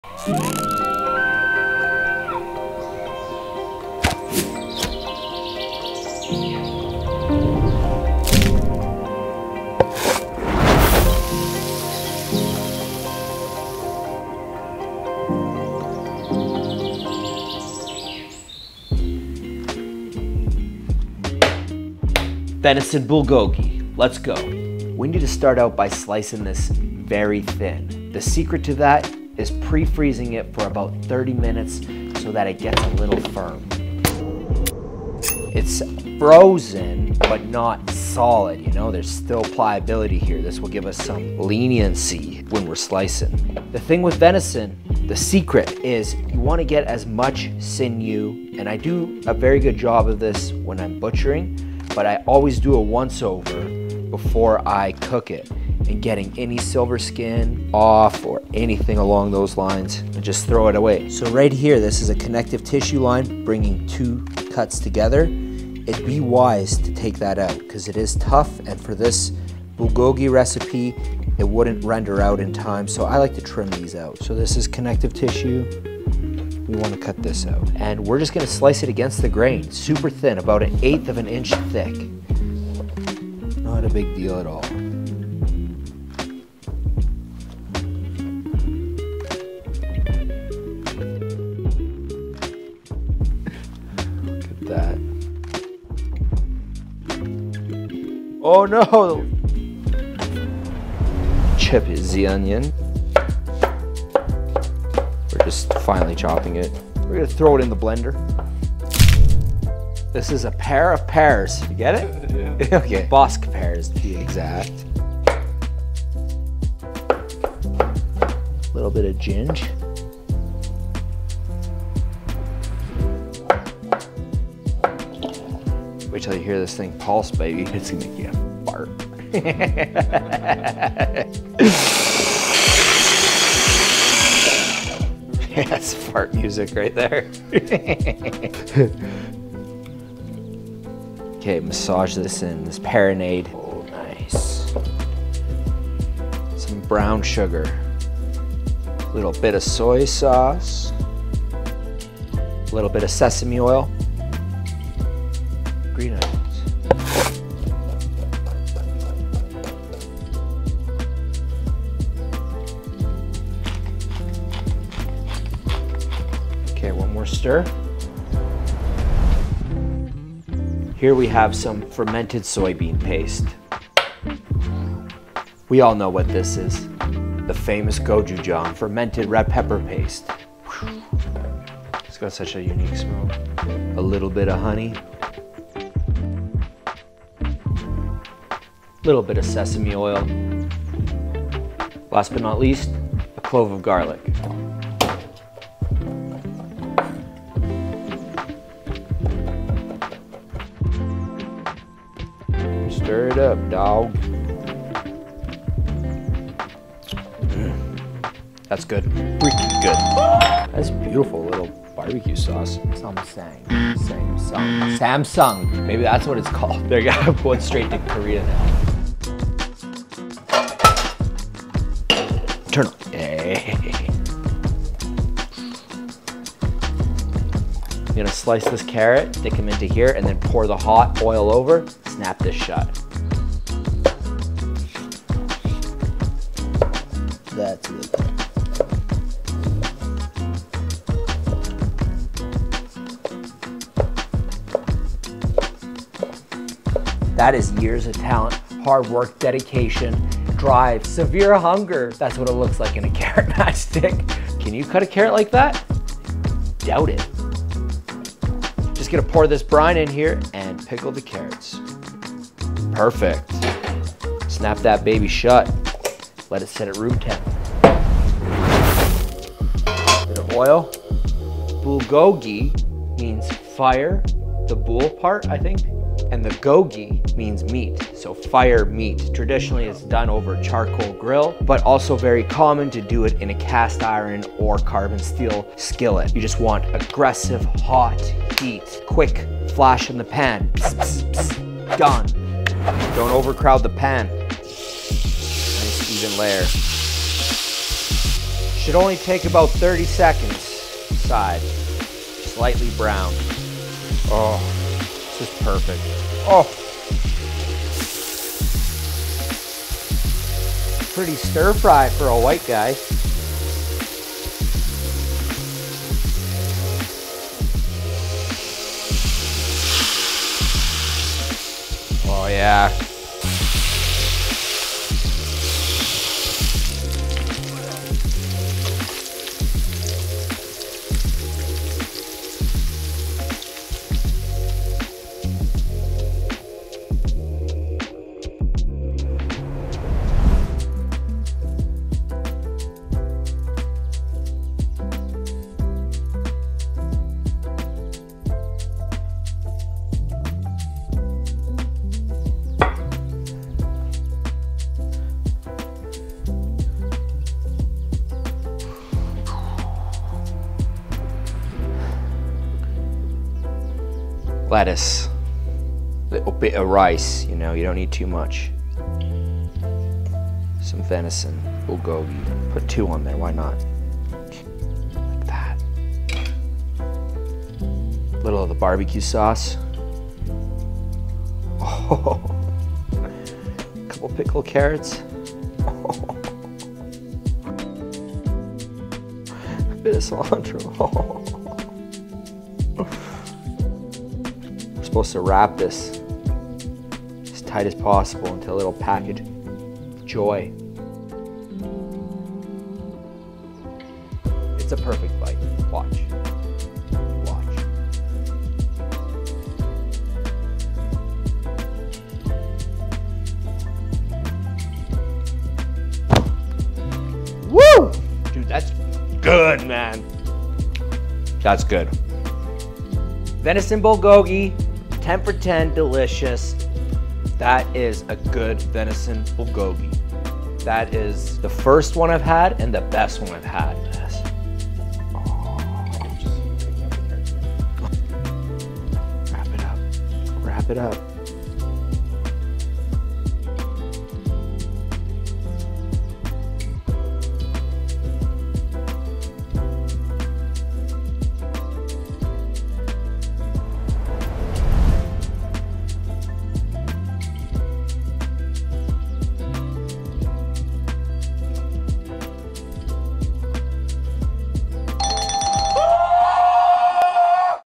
Korean BBQ Bulgogi... With VENISON??
Korean BBQ Venison Bulgogi with Wild Elk Meat Recipe 🥩🥘
Full Recipe:
VENISON BULGOGI
 • 3 pound venison round roast, silver skin removed and sliced thin

Marinade
 • 1 onion, chopped
 • 1 nub of ginger, sliced
 • 2 cloves garlic, crushed
 • 2 Bosc or Asian pears, chopped
 • 3 tbsp soy sauce
 • 2 tbsp brown sugar
 • 1 tbsp of sesame oil
 • 3 green onions, sliced

 ⁃ blend onion, pear, ginger, garlic together in food processed
 ⁃ Combine remaining ingredients and marinade in sliced venison for up to 2 hours

Korean BBQ Sauce
 • 2 tbsp fermented soy bean paste
 • 2 tbsp gochujang
 • 1 tbsp honey
 • 1/2 tbsp sesame oil
 • 1 large clove garlic, minced

 ⁃ combine all ingredients

Picked Carrots
 • 1 large carrot, julienned
 • 1/2 cup rice vinegar
 • 1 tbsp white wine or apple cider Vin
 • 5 tbsp sugar
 • 1 tsp salt
 ⁃ combine sugar, vinegars and salt, bring to a boil and pour over carrots. Let sit until room temp

Cooking
 ⁃ heat a cast iron or carbon steel pain and high heat, get the pan nice and hot with a drizzle of sesame oil
 ⁃ Spread a thin layer of the venison and quickly fry until golden brown or about 30 seconds a side
 ⁃ Work the venison in batches so that the pan doesn’t overcrowd
 ⁃ Garnish with sliced leeks or green onion and sesame seed

Assemble
 ⁃ lettuce
 ⁃ Sushi rice
 ⁃ Venison
 ⁃ BBQ sauce
 ⁃ Pickled carrot
 ⁃ Cilantro (optional)

Products In This Video:
🍳MERTEN & STORK Carbon Steel Induction Skillet
🥘LODGE Cast-Iron Skillet
🧂Diamond Crystal Salt (the BEST cooking salt)

Gear Used to Make This Video:
Camera And Lens
Tripod Head

Table of Contents:
0:00 - Intro
0:18 - Cut The Elk
1:40 - Marinade
2:49 - BBQ Sauce
3:39 - Pickle the Carrots
4:28 - Pretty Stir Fry for a White Guy
5:40 - EAT